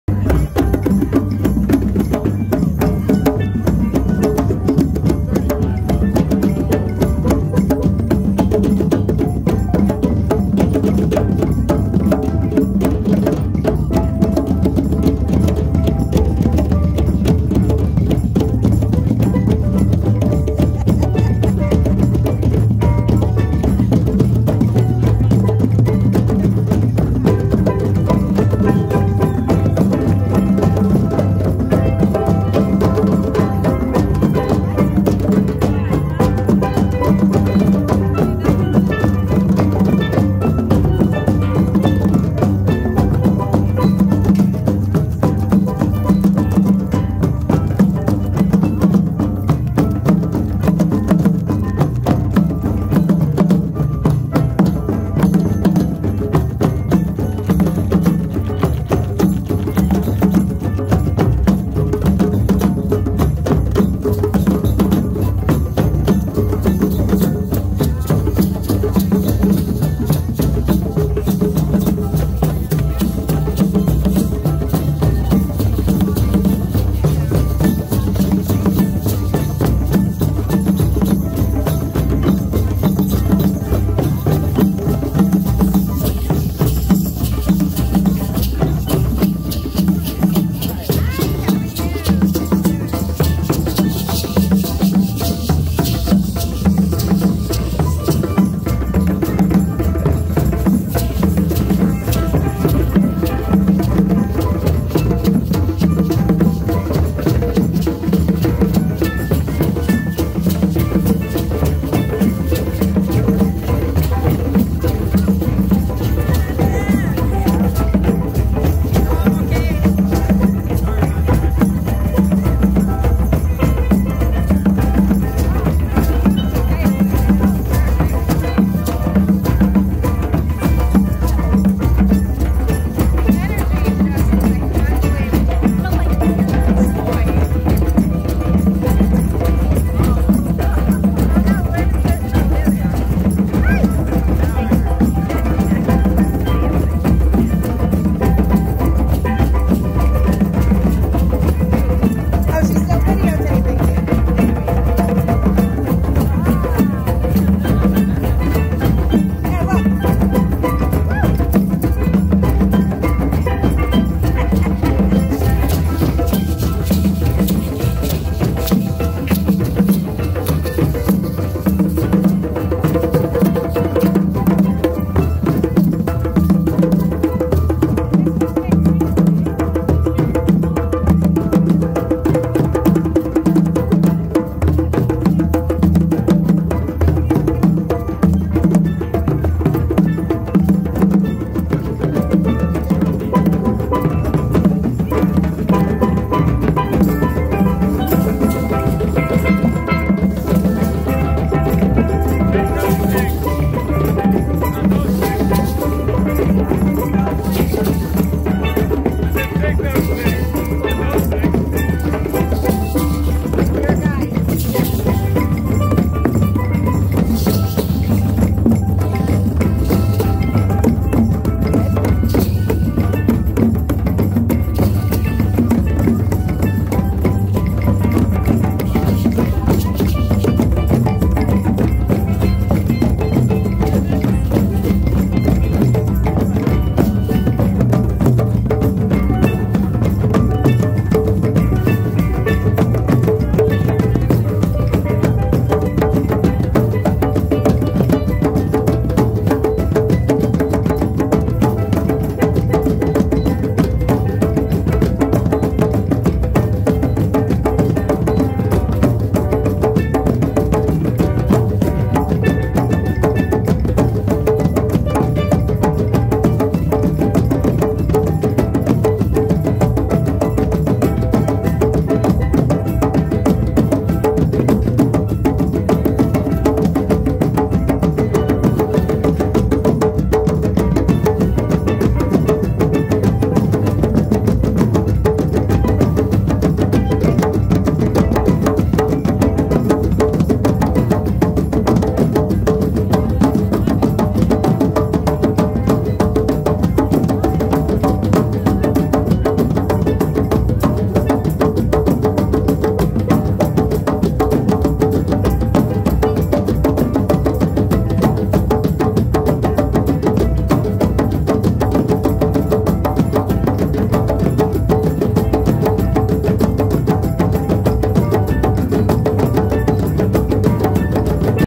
Free Culture Zone Kickoff Drum Circle
Join in the merriment we all felt on the opening day of drum season by the beach! Facilitated by Barbara Mele and Evans W Chigounis. Our drum circle happens along the boardwalk where Ocean Grove meets Asbury Park.

Recorded by Touch Mother Earth Productions
to see how we bring the Outside IN.